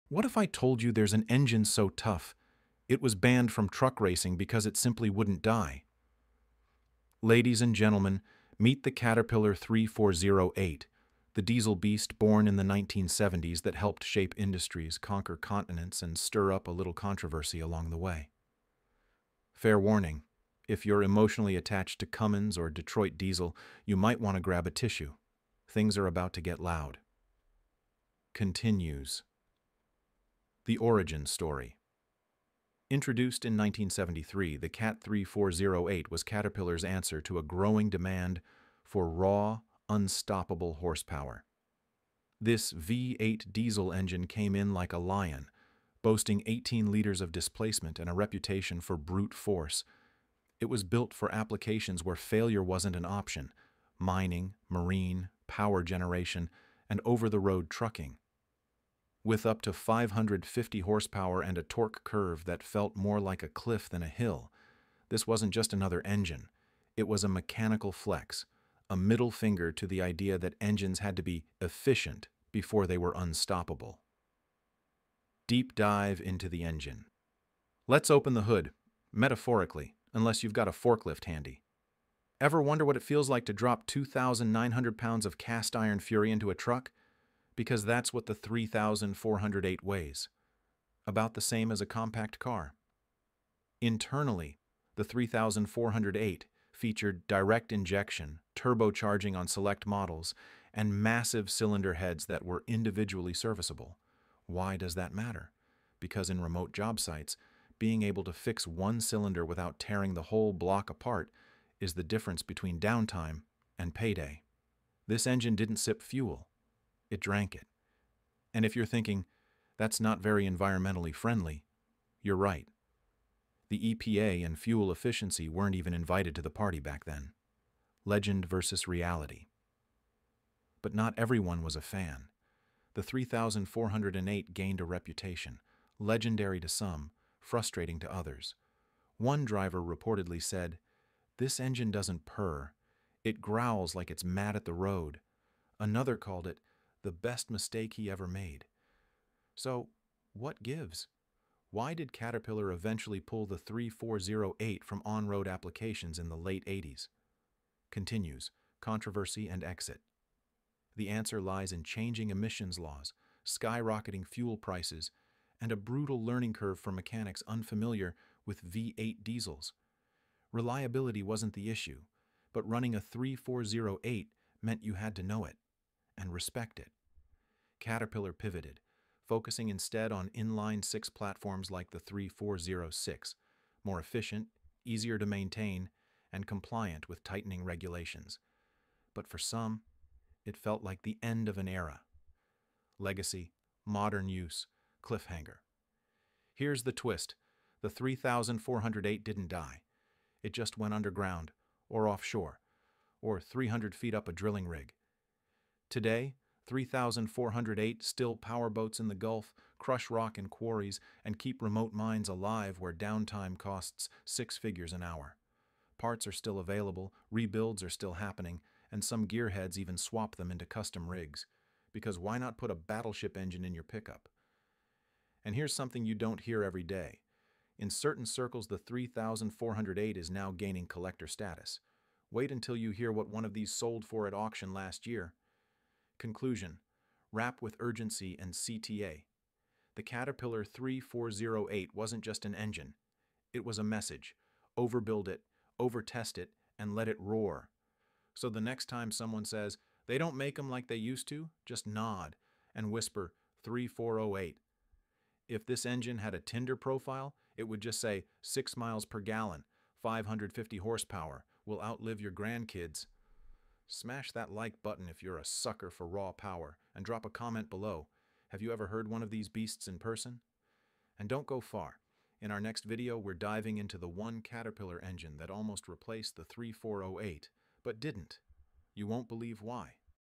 Caterpillar 3408 Engine Explained | Power, Performance & Facts
Discover everything you need to know about the Caterpillar 3408 engine in this powerful and informative speech. Whether you're a diesel enthusiast, heavy machinery expert, or just curious about iconic engines, this deep dive into the Caterpillar 3408 is packed with valuable insights. Learn about its legendary V8 engine design, impressive horsepower, and unmatched durability that made it a favorite in industrial, marine, and truck applications. The speaker breaks down its technical specifications, common maintenance tips, and real-world performance, making it perfect for mechanics, operators, or engineering students. This speech is not just about specs—it's about the legacy of a machine that helped shape industries.

Why Watch:
This speech gives a rare, detailed look at one of Caterpillar’s most iconic engines. If you love engines or are in the heavy-duty field, this is an unmissable piece of content.